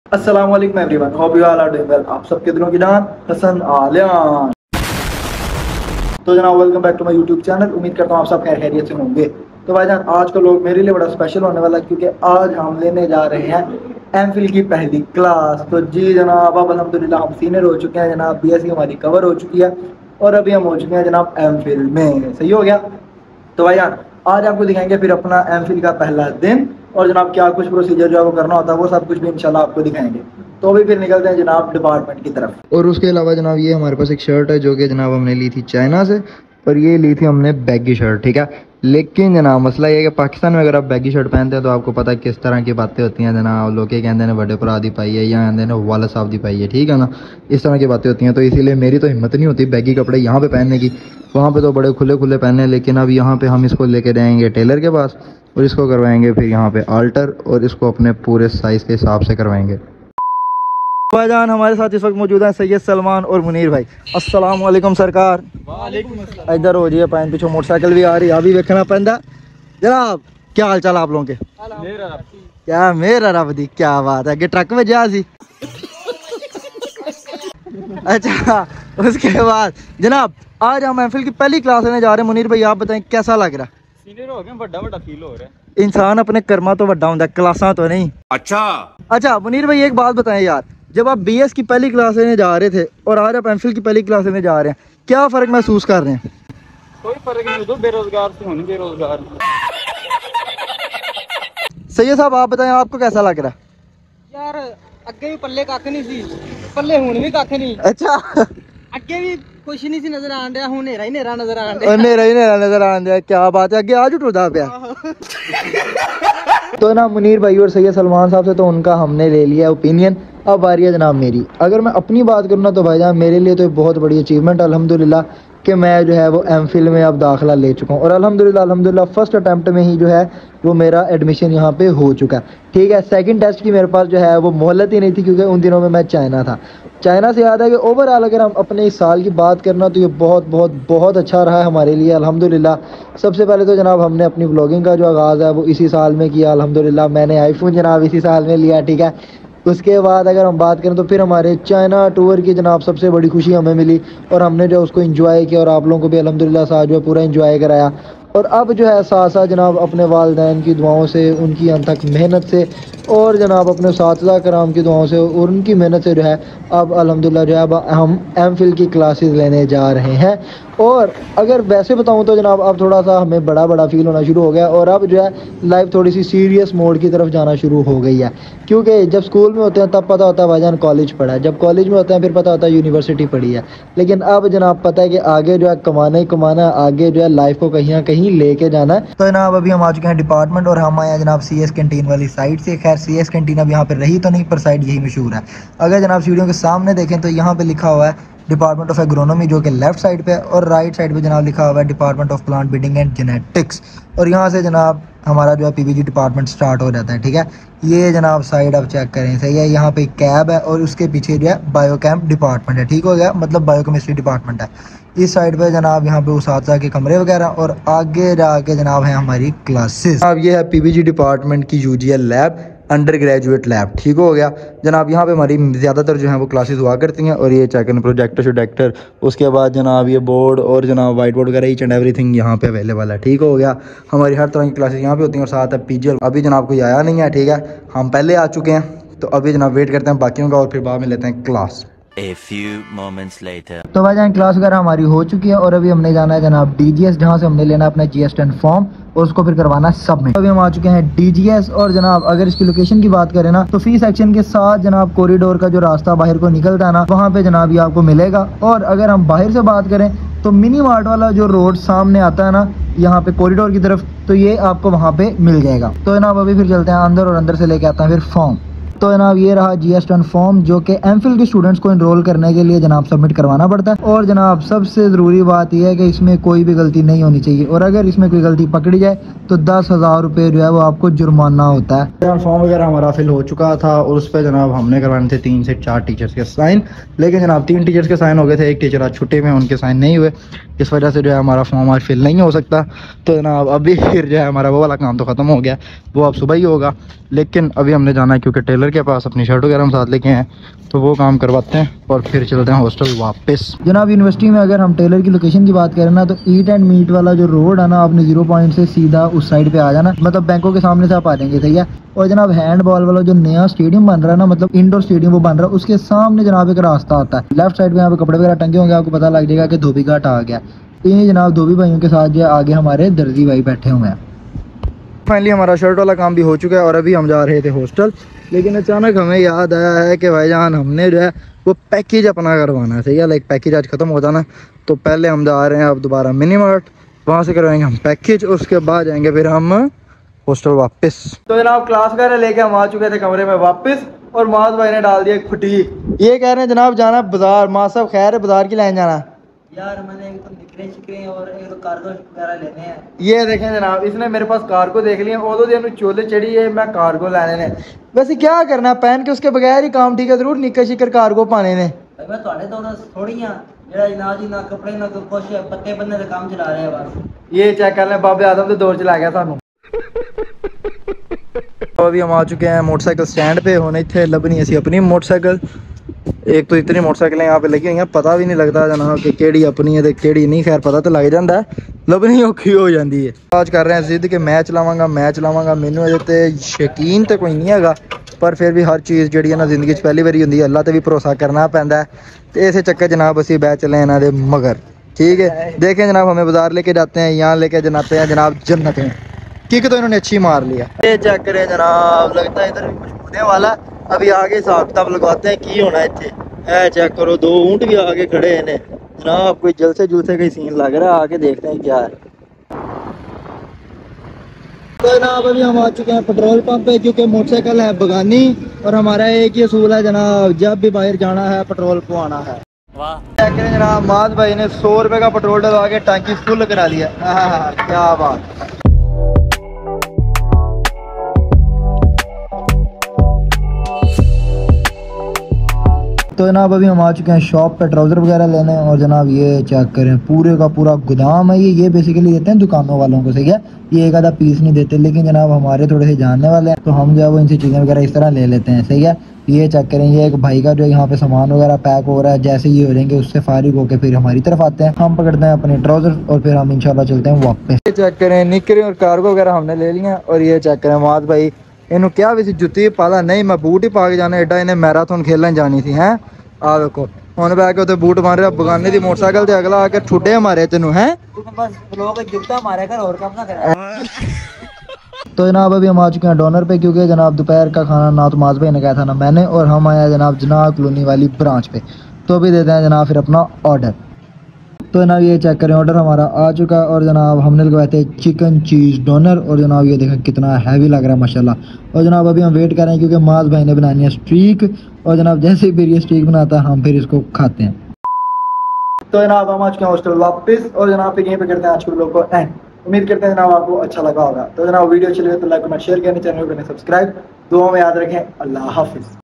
Mphil Ki Pehli Class Attend Ki Aj😍|Muaz Bhai ki Shopping|UAF|Faisalabad|Vlog#139
This vlog is about attending first class of mphil degree and Shopping Of Muaz.